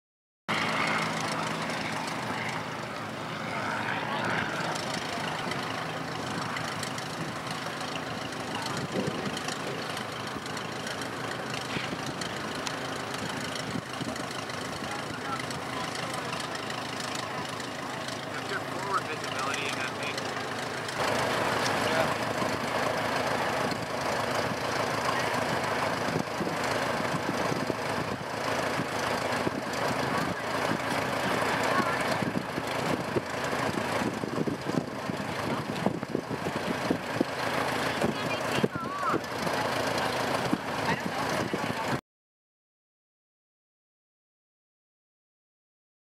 Short SC.7 Skyvan "Flying Shoebox"
A 1978 SHORT BROS. & HARLAND SC-7 SER 3 VAR 200 at the 2015 Summerside, Prince Edward Island airshow. This aircraft was manufactured by Short Brothers of Belfast, Northern Ireland. They are used mainly for short-haul freight and skydiving.